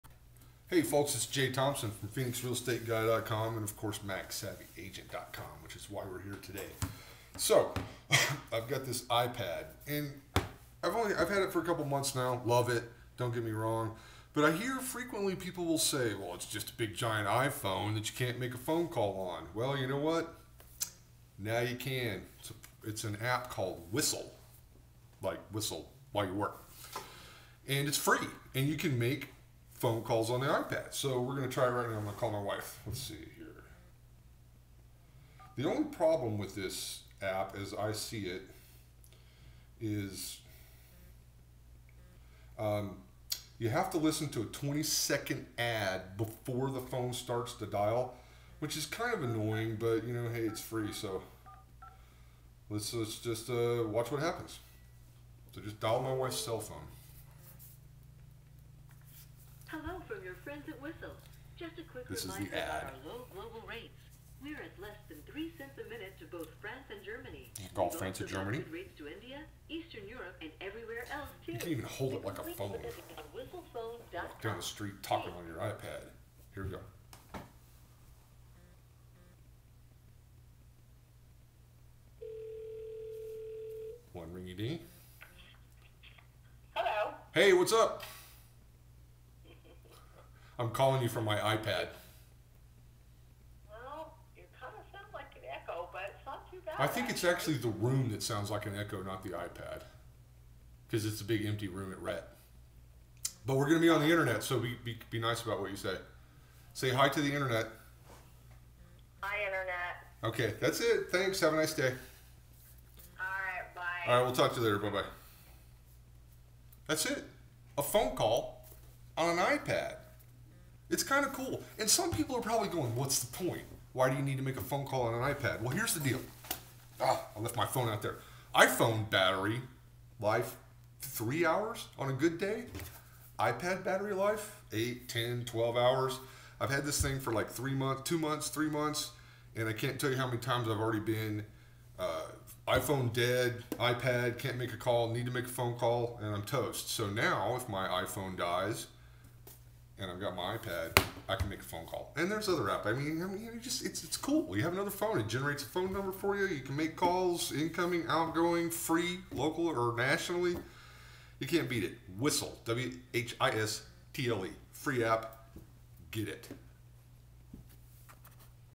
Make a phone call from your iPad!
"An iPad is just a big giant iPhone -- that you can't make a phone call with..." Well not any more! Using the FREE ap Whistle, you can now make calls from your iPad!